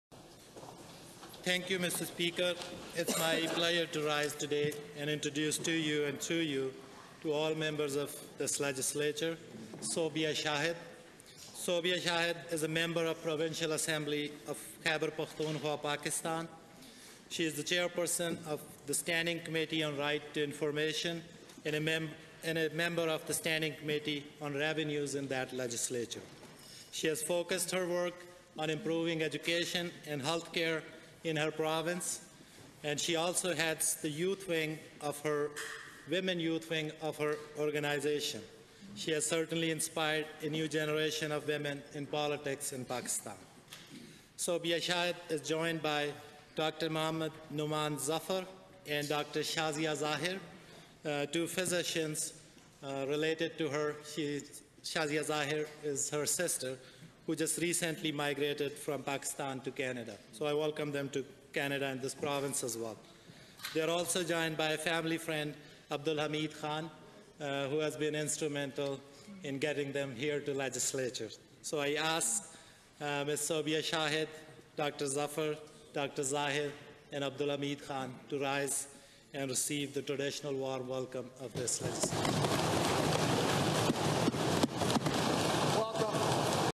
Honourable Irfan Sabir introduces visitors in the Alberta Legislature
Honourable Irfan Sabir introduces Sobia Shahid, Dr. Muhammad Nauman Zafar, Dr. Shazia Zahir and Abdul Hamid Khan, visitors to the Alberta Legislature on June 6,2016.